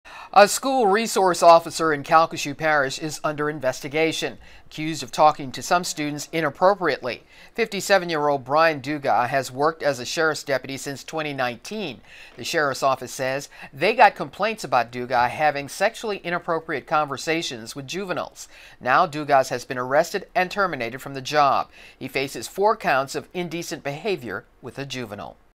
CPSO: School resource officer fired and arrested after inappropriate conversations with children
A Calcasieu school resource officer was fired and arrested on allegations of inappropriate behavior with children.